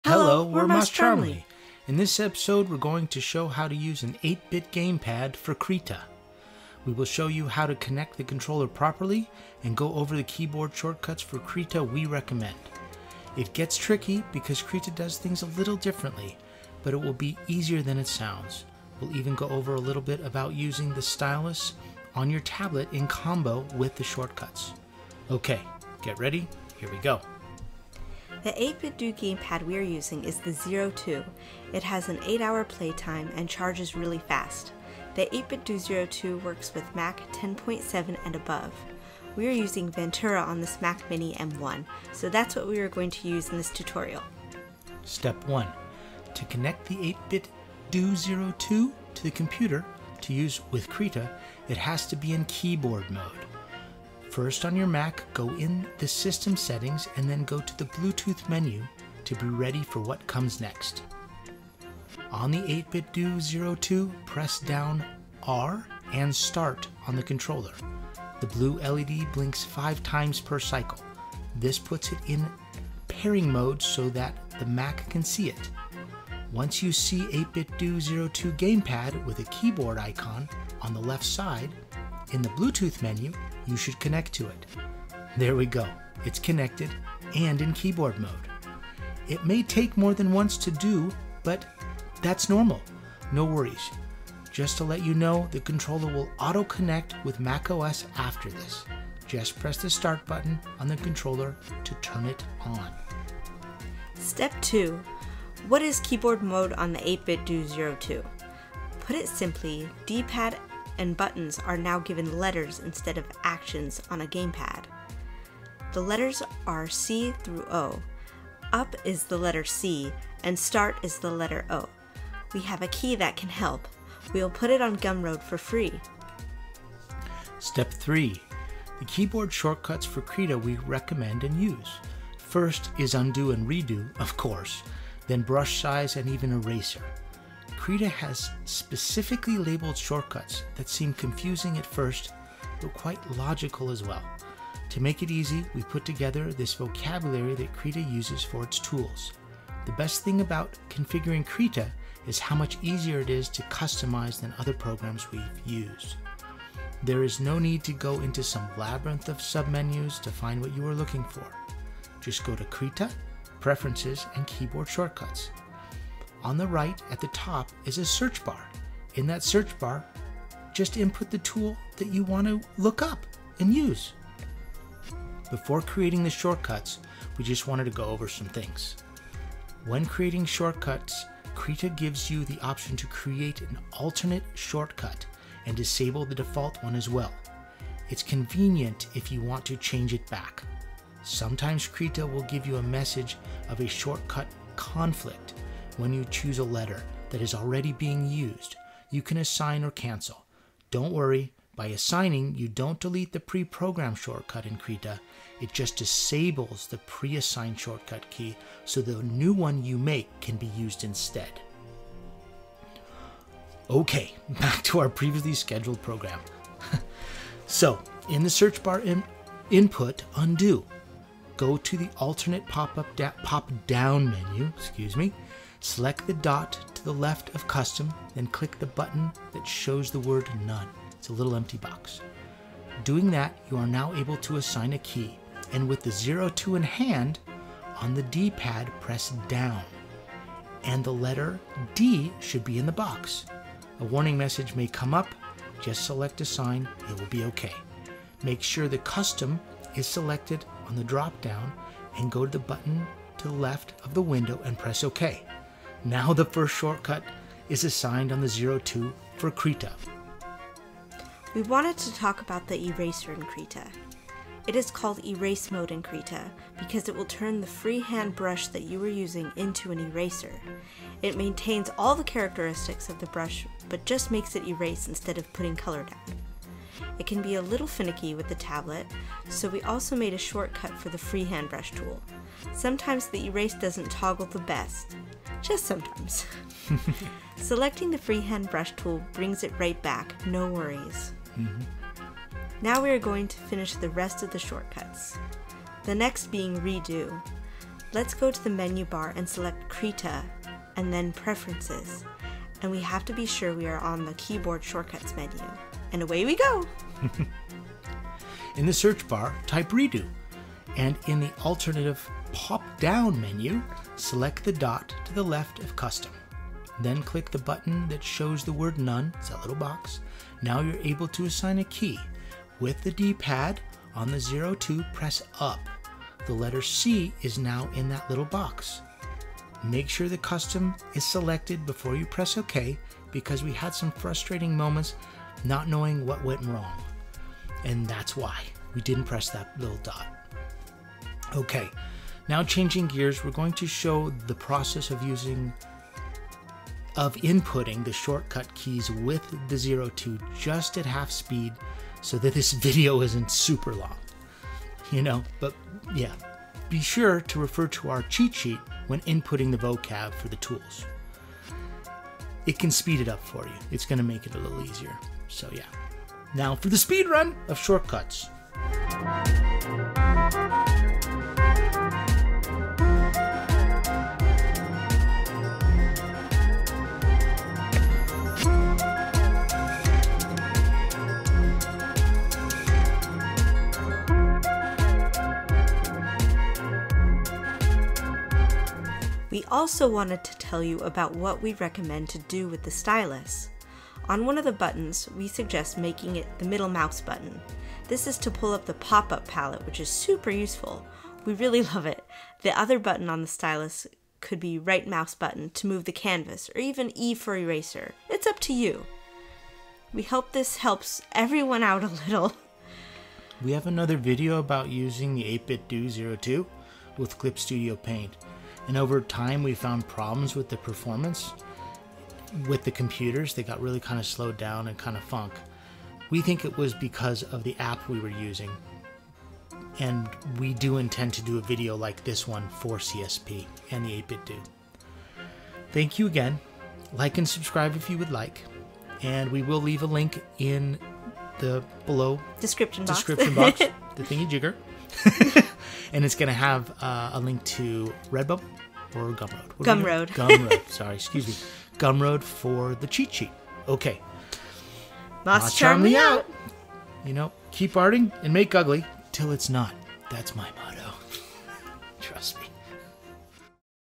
Krita & 8BitDo Zero2 (Shortcut Keys Tutorial)
Hello, MossCharmly here.  Today we're showing how to input shortcut keys on Krita with the 8BitDo Zero2.  We hope this helps you out and makes working on Krita more efficient for you.